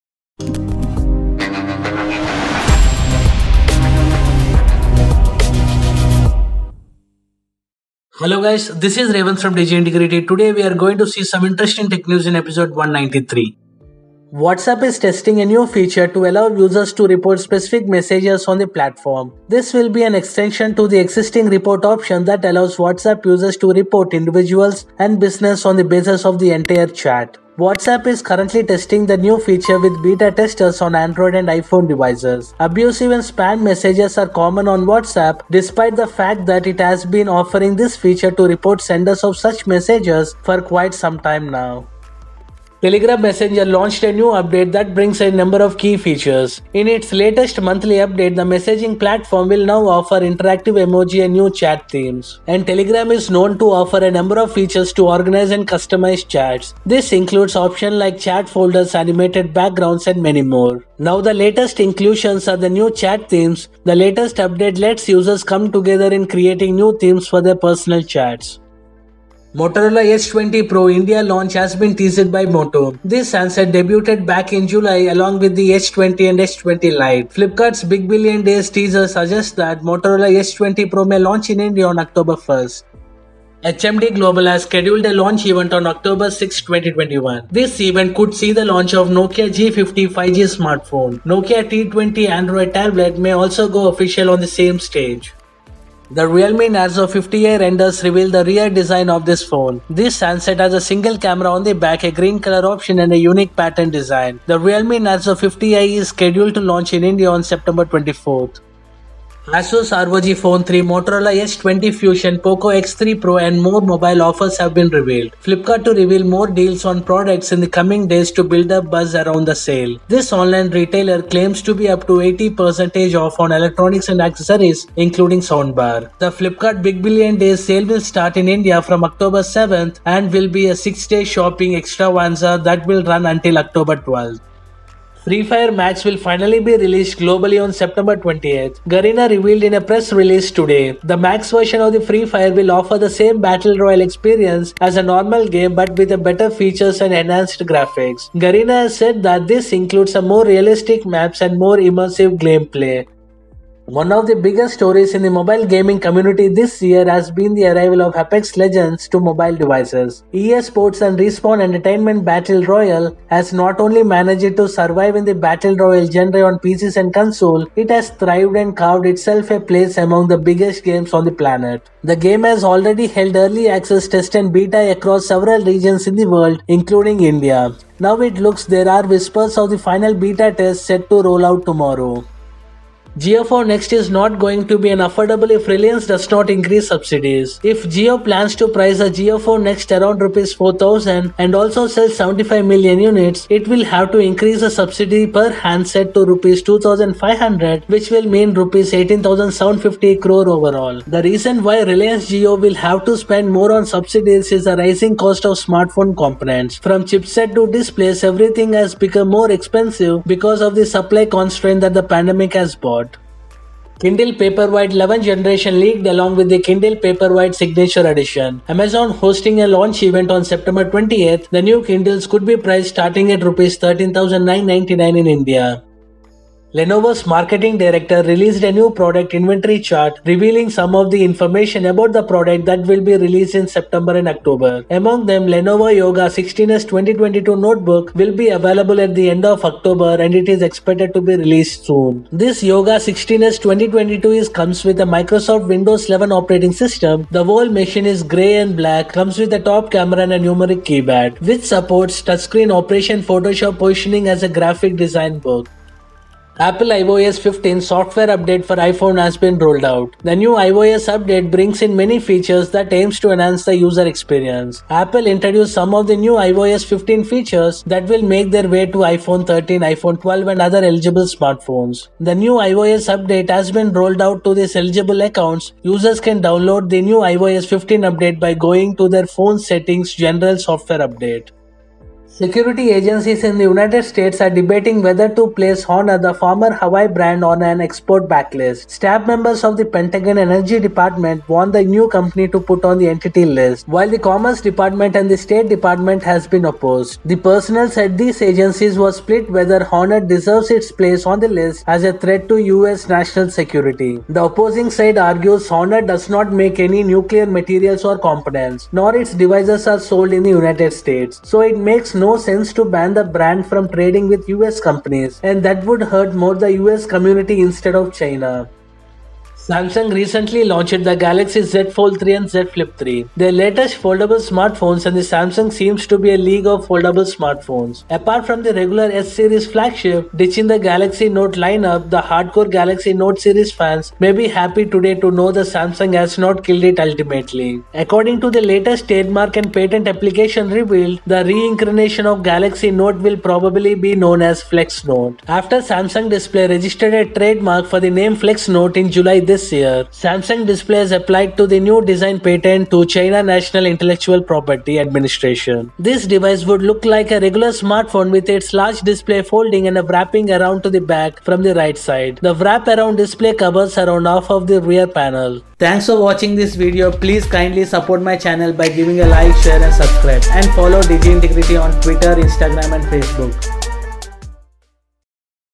Flipkart BBD Sale📅😍, Jiophone Next Subsidy, Edge 20 Pro Launch, Apex Legends & Free Fire Max-DIN193
Video Highlights
(0:00) : Introduction
(0:14) : WhatsApp Testing a Feature on Android, iOS to Let You Report Specific Messages, Instead of Entire Chats
(0:42) : Telegram update brings Interactive Emoji and New Chat Themes
(1:09) : Motorola Edge 20 Pro India launch teased officially
(1:23) : Nokia teases launch event on October 6th
(1:36) : Realme Narzo 50i render reveals rear design ahead of this week’s India launch
(1:49) : Flipkart Big Billion Days sale starts October 7
(2:15) : Free Fire MAX will be released globally on Sept. 28
(2:33) : Apex Legends Mobile: Final Beta Test Could Come to India and Other Regions Tomorrow
(3:03) : JioPhone Next Will Require Reliance to Spend More on Subsidy
(3:33) : Amazon hosting a launch event on September 28
(3:46) : Lenovo officially released YOGA 16s 2022
(4:16) : iOS 15 Update for iPhone 12, iPhone 11 and Older iPhones Rolled Out
(4:42) : US agencies consider a ban for newly-independent Honor
(5:19) : Samsung Galaxy Flex Note With Fold-Out Display and S Pen Support Could Be in the Works, Patent Reveals
(6:05) : Thanks for watching

iQOO Z5 🇮🇳 Launch, Mi 11 Lite 5G, Samsung Galaxy S22, Samsung A73 5G, HP Sold Most PCs 2021-DIN191